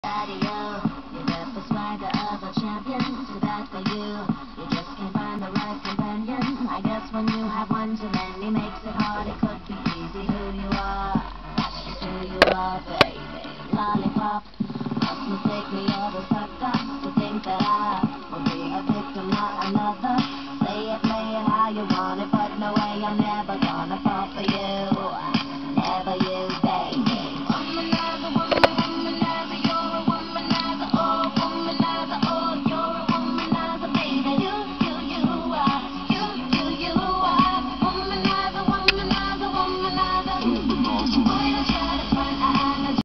Laurette met en scène les playmobils 2
Le rapport avec Britney n'est pas évident et hélas c'est trop sombre, mais elle n'a que 9 ans et débute dans la mise en scène ;-)